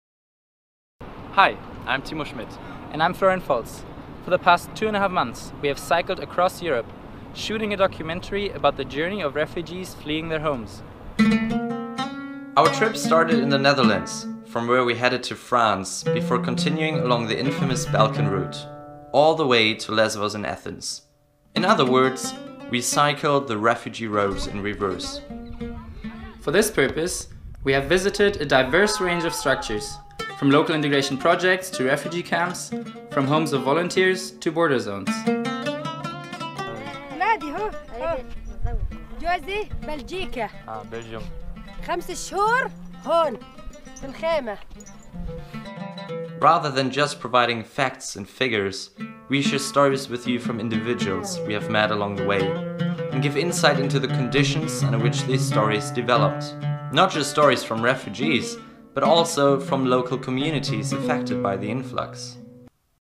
Refugee Roads - Crowdfunding Teaser from 2016
Do you remember our crowdfunding campaign in 2016? Through the platform CineCrowd, RefugeeRoads reached 141 people who believed in this project after only seeing this fairly raw video that we edited ourselves. Many of those early supporters did not even know us in person. Back then we had just returned from Athens and we urgently needed some financial support to have any chance of making a documentary with our numerous hours of video material. The problem: besides Timo Schmidt and me, no one was aware yet of what we were so sure of already: the people we encountered that summer had a right to be heard and their stories that we collected had to be shared.

It was only through this successful crowdfunding campaign that we were able to take the next step in the project and start the post-production of our first episode. After a huge behind-the-scenes journey over the last three years that brought additional support to our production, we are now immensely grateful to be able to bring the stories of Refugee Roads to all of you.